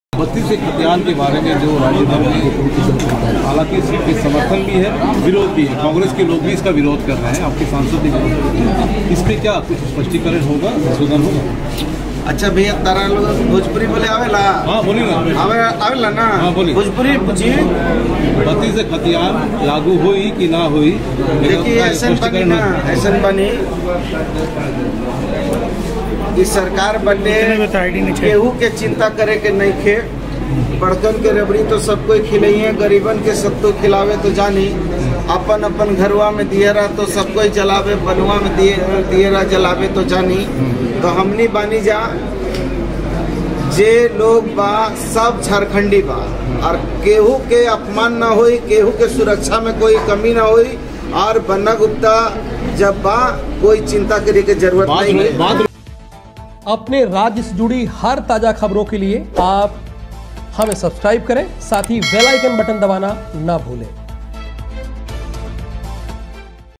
1932 खतियान पर भोजपुरी में बोले मंत्री Banna Gupta, आप भी देखें इस वीडियो को.. || Health Minister
1932 खतियान पर भोजपुरी में बोले मंत्री Banna Gupta, आप भी देखें इस वीडियो को.. || Health Minister ||